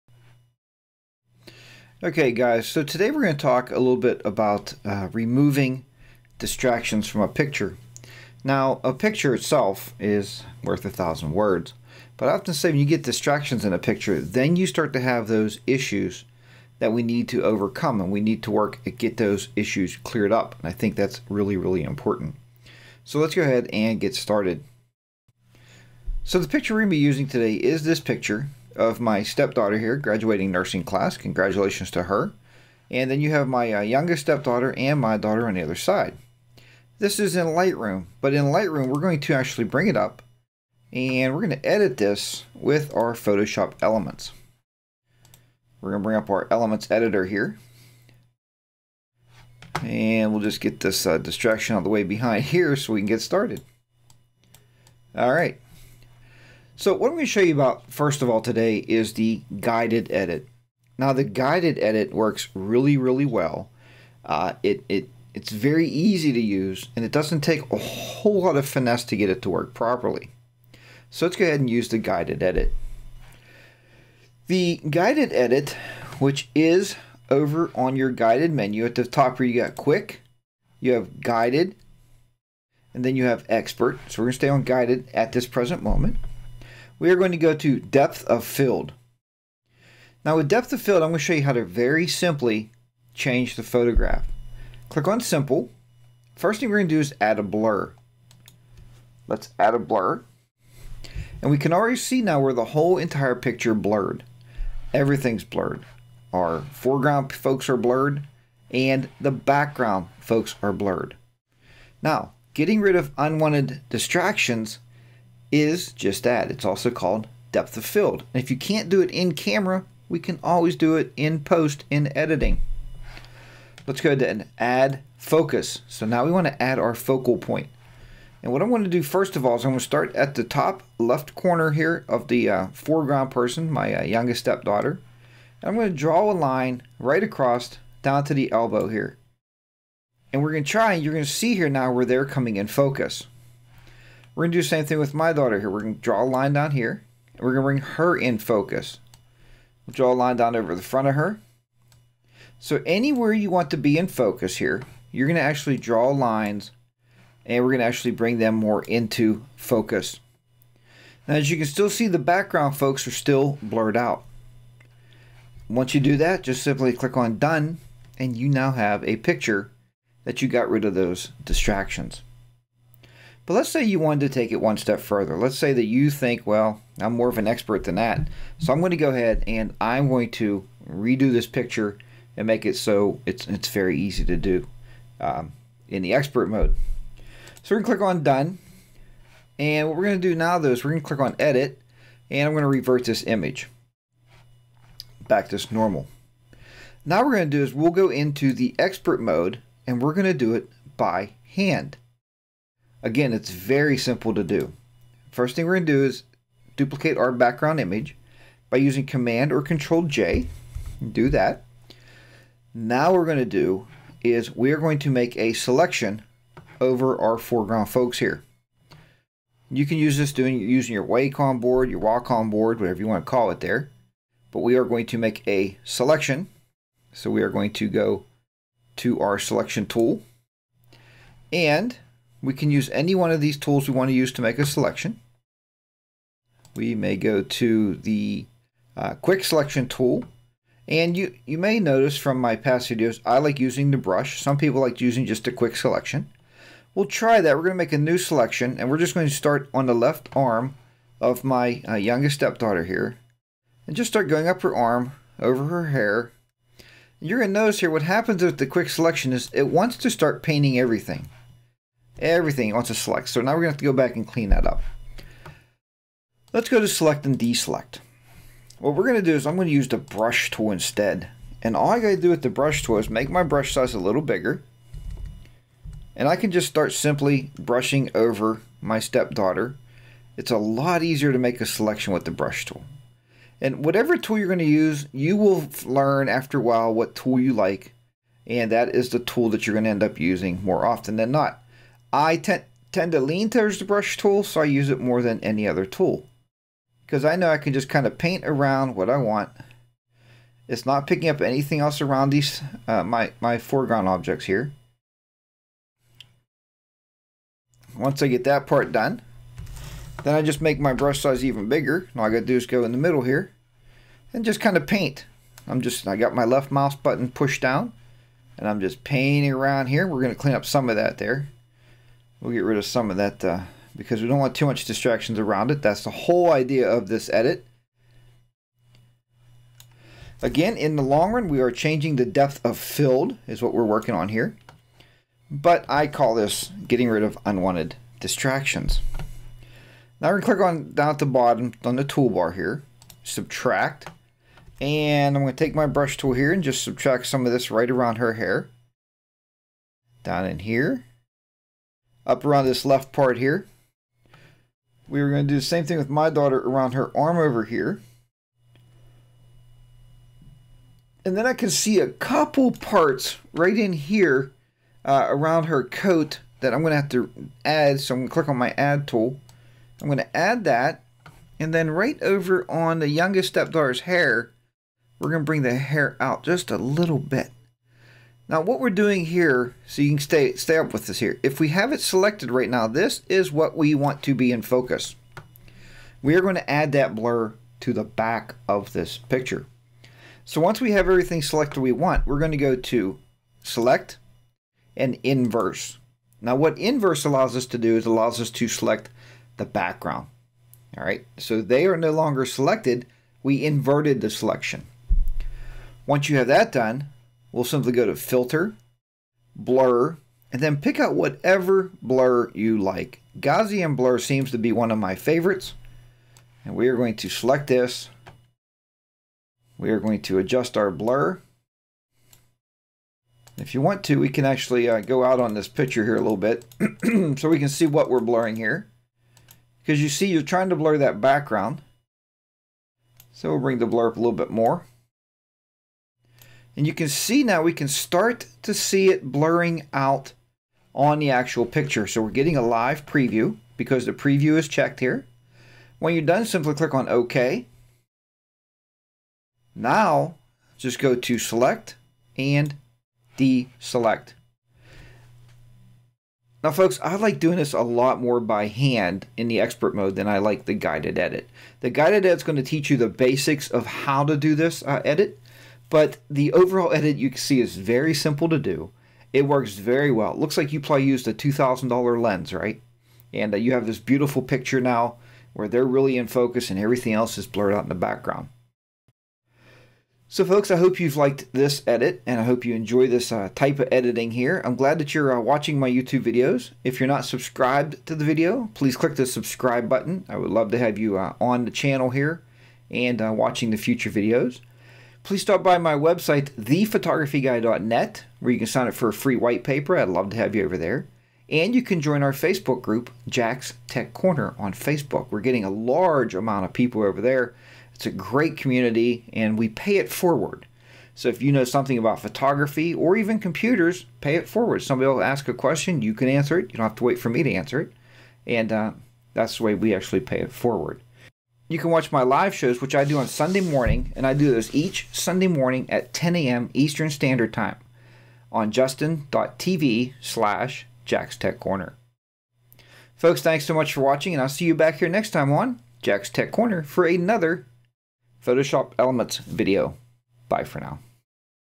Photoshop Elements Eliminating Distractions
Many times you will take a picture and after getting into your editor you wished that you would have watched the background a little better. This video will assist you to clean up those distractions.